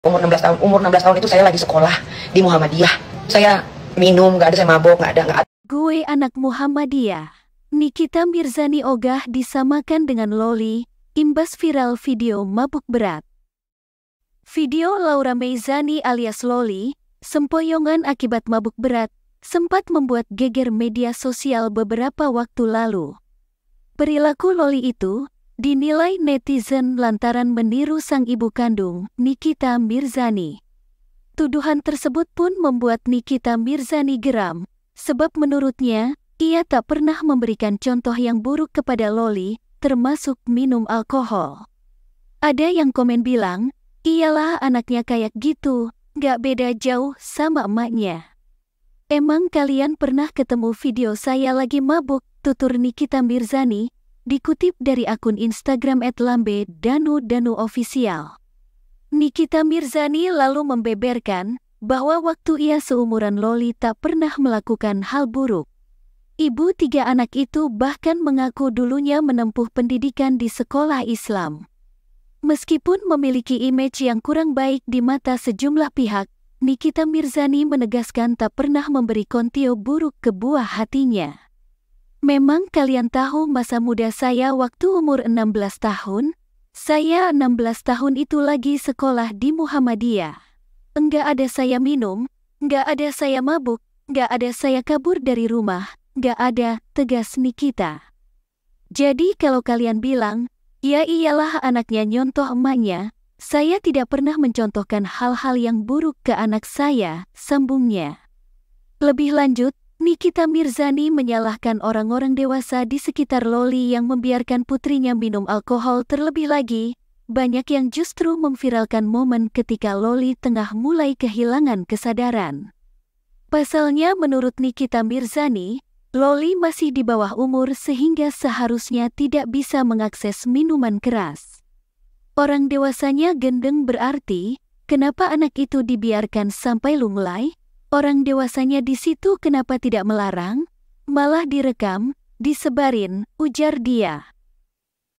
“Gue Anak Muhammadiyah” Nikita Mirzani OGAH Disamakan Dengan Lolly, Imbas Viral Video Mabuk
Perilaku Lolly itu, dinilai netizen lantaran meniru sang ibu kandung, Nikita Mirzani.

Tuduhan tersebut pun membuat Nikita Mirzani geram, Sebab menurutnya, ia tak pernah memberikan contoh yang buruk kepada Lolly, termasuk minum alkohol.

"Ada yang komen bilang, 'Iyalah anaknya kayak gitu, nggak beda jauh sama emaknya'. Emang kalian pernah ketemu video saya lagi mabuk?" tutur Nikita Mirzani, dikutip dari akun Instagram @lambe danu2 official.

Nikita Mirzani lalu membeberkan, bahwa waktu ia seumuran Lolly tak pernah melakukan hal buruk. Ibu tiga anak itu bahkan mengaku dulunya menempuh pendidikan di sekolah Islam.

Meskipun memiliki imej yang kurang baik di mata sejumlah pihak, Nikita Mirzani menegaskan tak pernah memberi contio buruk ke buah hatinya.

"Memang kalian tahu masa muda saya waktu umur 16 tahun? Saya 16 tahun itu lagi sekolah di Muhammadiyah. Enggak ada saya minum, nggak ada saya mabuk, nggak ada saya kabur dari rumah, nggak ada," tegas Nikita.

"Jadi kalau kalian bilang, 'Ya iyalah anaknya nyontoh emaknya', saya tidak pernah mencontohkan hal-hal yang buruk ke anak saya," sambungnya.

Lebih lanjut, Nikita Mirzani menyalahkan orang-orang dewasa di sekitar Lolly yang membiarkan putrinya minum alkohol Terlebih lagi, banyak yang justru memviralkan momen ketika Lolly tengah mulai kehilangan kesadaran.

Pasalnya menurut Nikita Mirzani, Lolly masih di bawah umur sehingga seharusnya tidak bisa mengakses minuman keras.

"Orang dewasanya gendeng berarti, kenapa anak itu dibiarkan sampai lunglai? Orang dewasanya di situ kenapa tidak melarang? Malah direkam, disebarin," ujar dia.